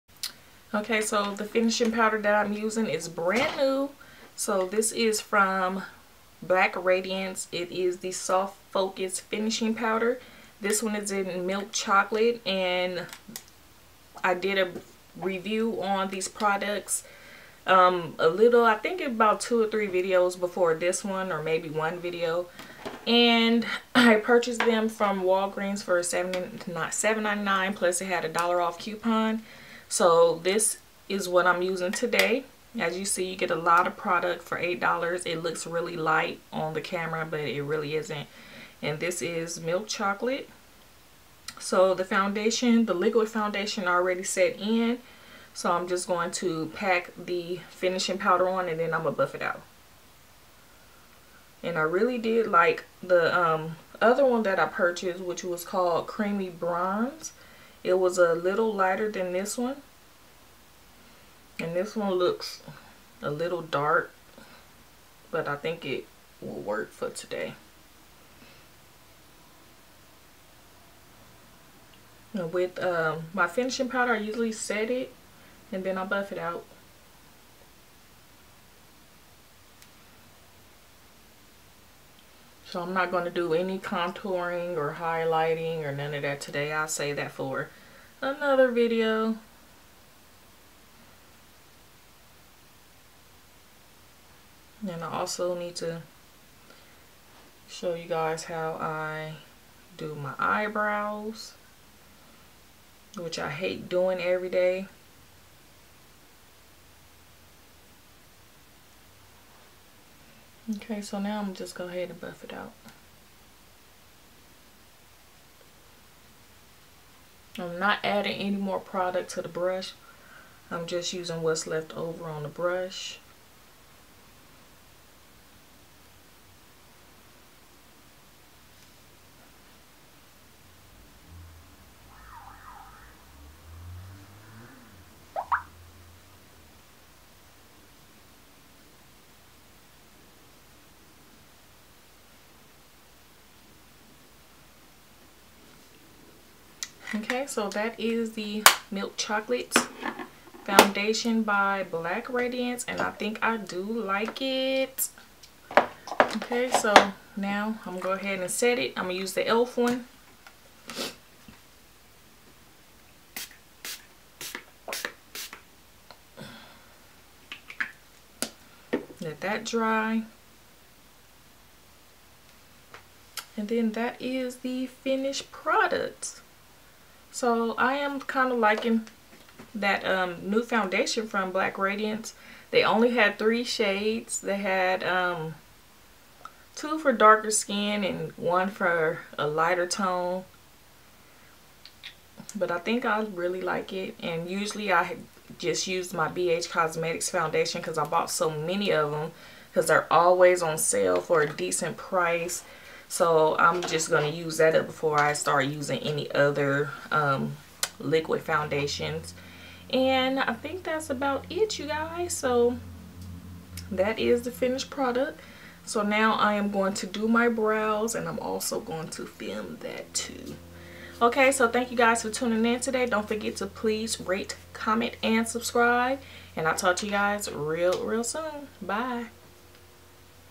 Black Radiance Soft Focus Finishing Powder Demo
HERE IS A QUICK FOUNDATION ROUTINE FOR FILMING PURPOSES. I USE BH COSMETICS FOUNDATION IN DEEP COCOA AND BLACK RADIANCE PRESS POWDER IN MILK CHOCOLATE.